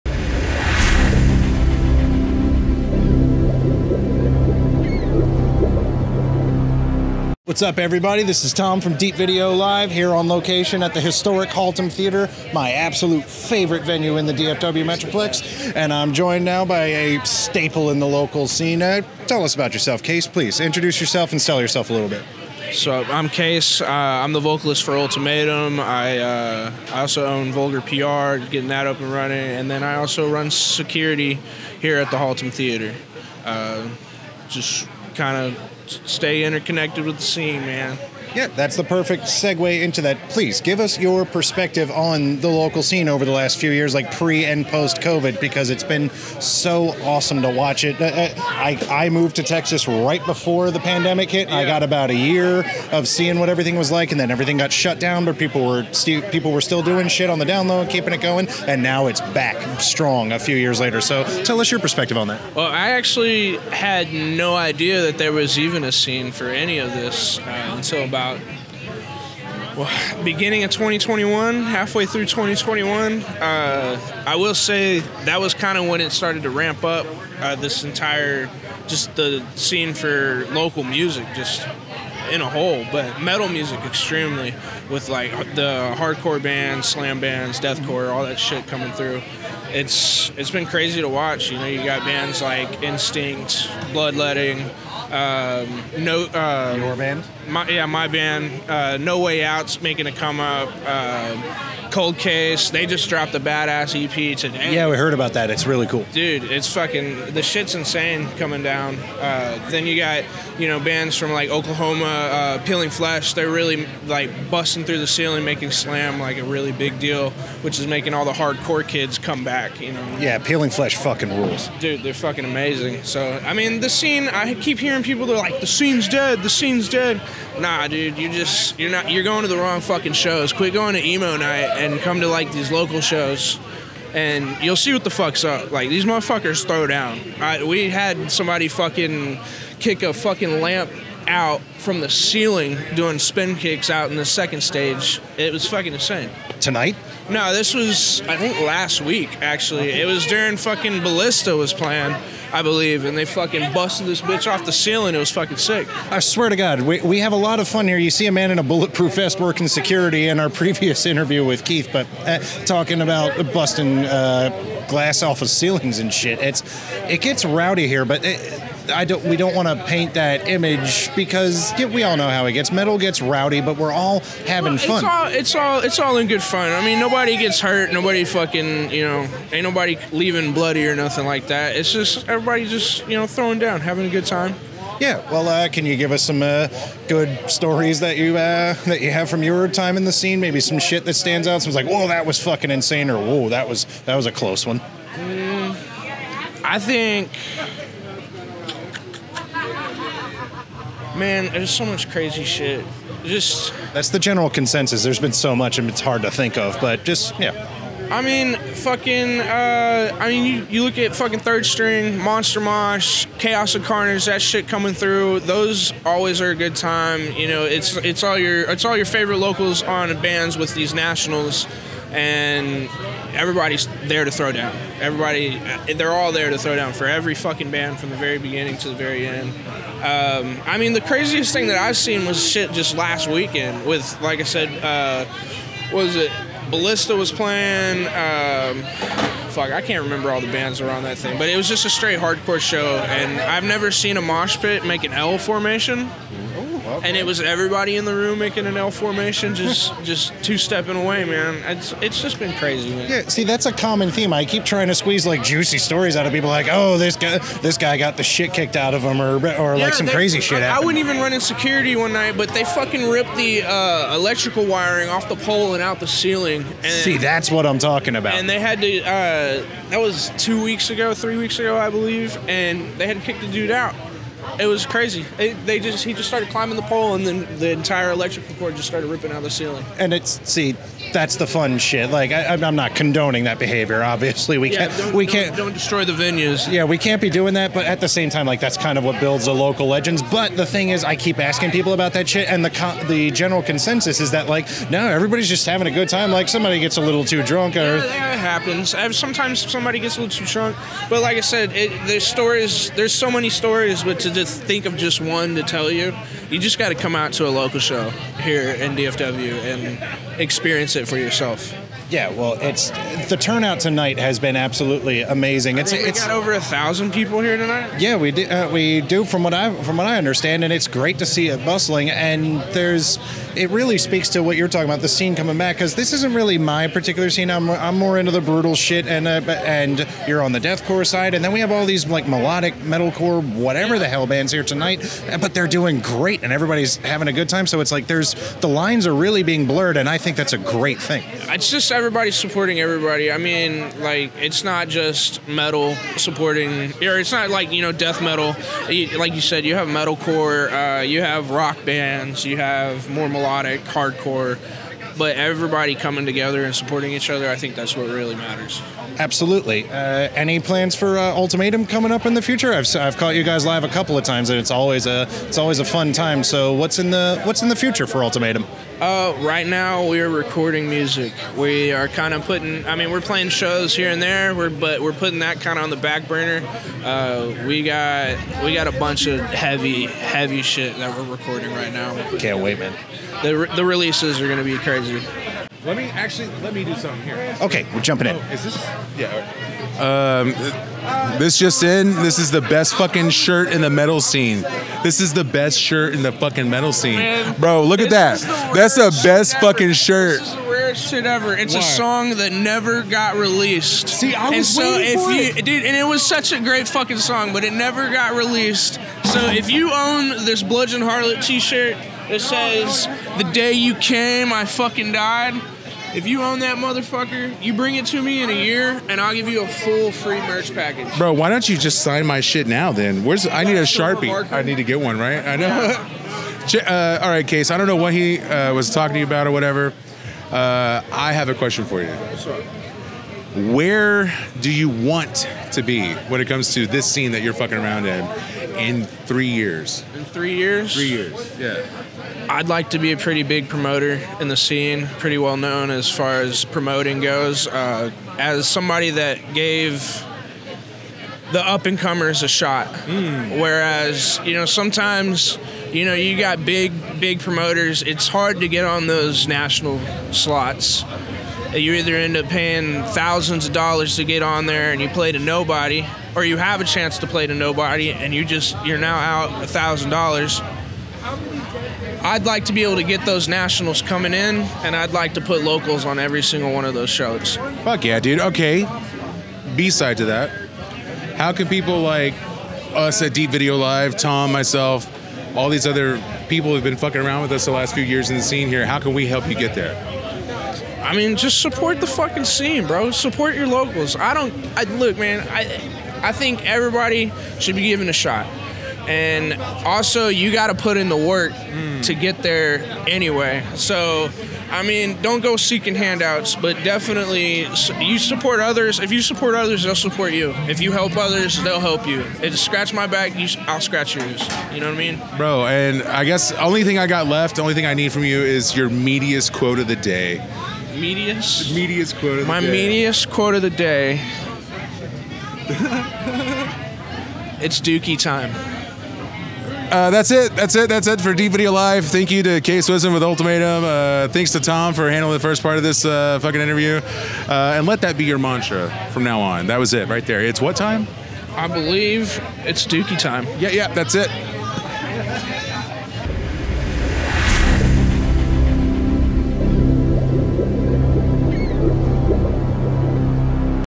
Kase Wisdom vocalist of
Tom and Jason popped over to the Havoc at the Haltom metal show in February. Check out this interview with Kase Wisdom of Ultimatum!